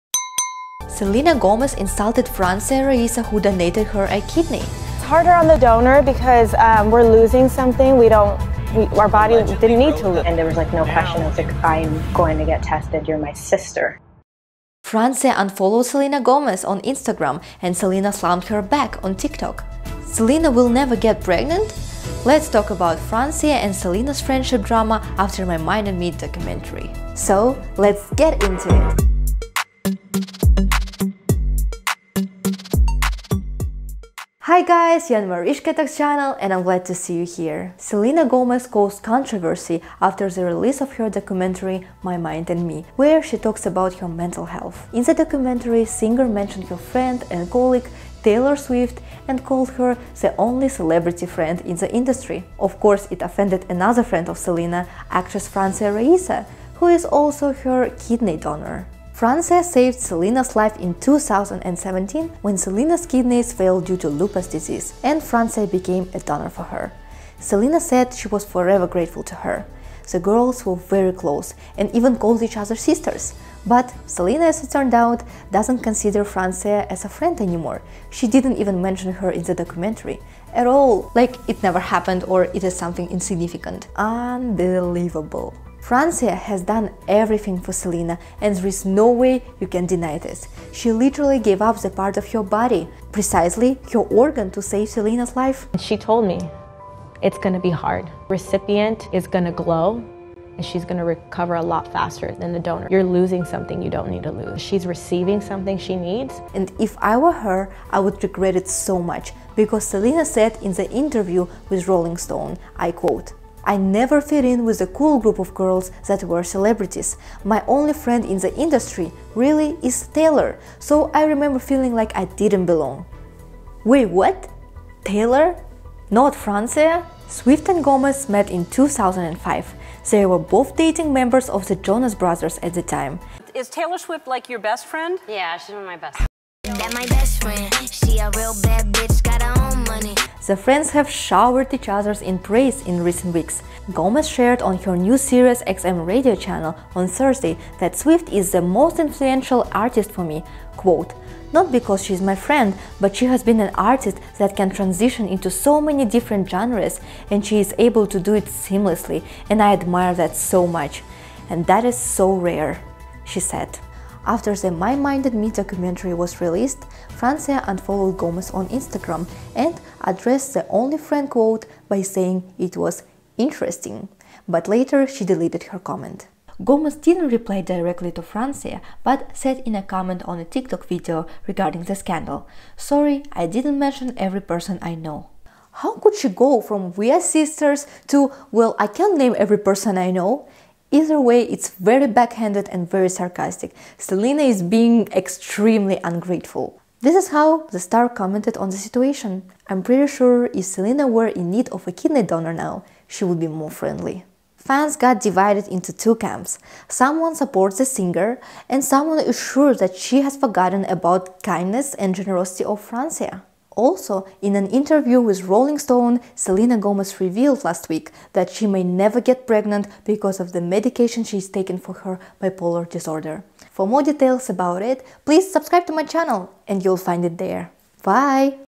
SELENA GOMEZ and FRANCIA RAISA friendship DRAMA
Hi Guys, we are so lucky cuz' the news around Selena Gomez does not stop coming and it's so thrilling. Selena's new documentary "MY MIND & ME" which was recently released, uncovered and provoked an issue between Selena Gomez and her EX-Friend, Francia Raisa.

00:00 - Selena and Francia's feud
00:28 - Intro
00:37 - "Taylor is the only celebrity friend in the industry"
00:54 - Francia Raisa saved Selena's life
01:13 - From sisters to acquaintances
01:35 - Francia Raisa's interview about the surgery
01:53 - Selena's only true friend is Taylor
02:44 - Francia unfollowed Selena and Selena responded
02:55 - "Interesting" comment
03:02 - "Sorry I didn't mention every person I know" - Selena
03:24 - Fans' opinion
03:33 - Selena may never have children